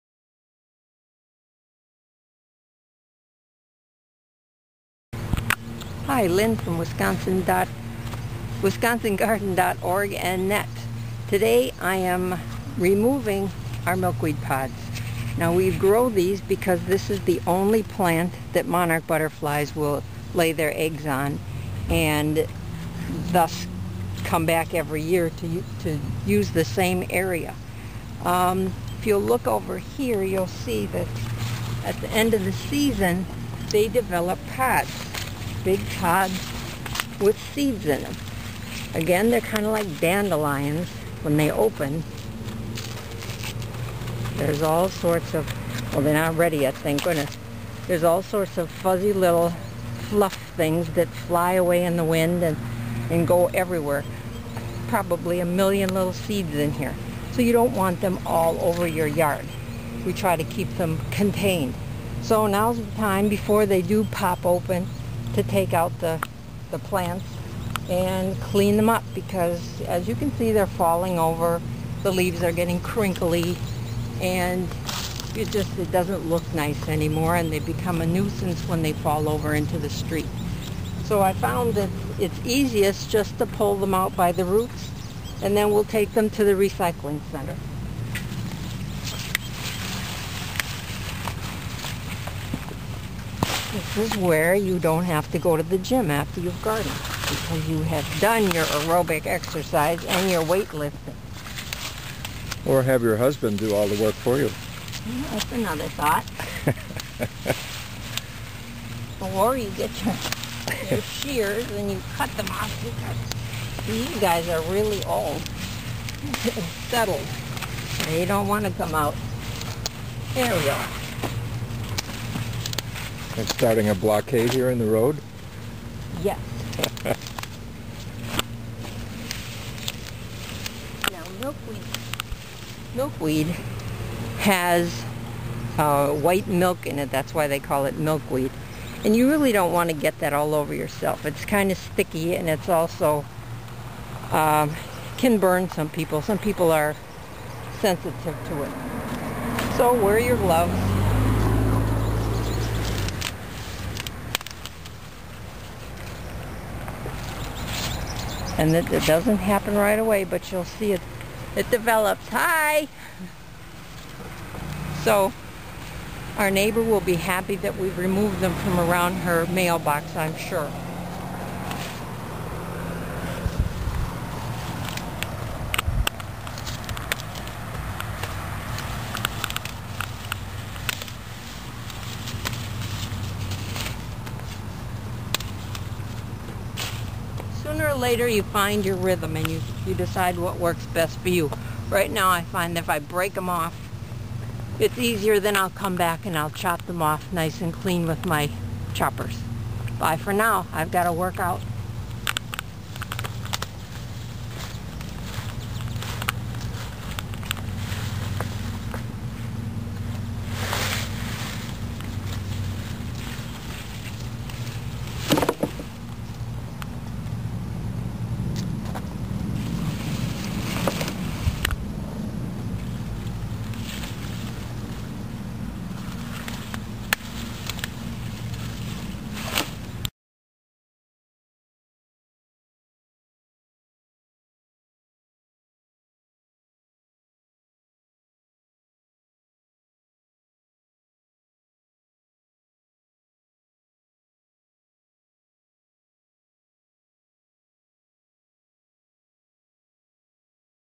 Butterfly Milkweed - Wisconsin Garden Video Blog 41.wmv
While we love our Monarch Butterflies, our milkweed patches need to be controlled before they burst.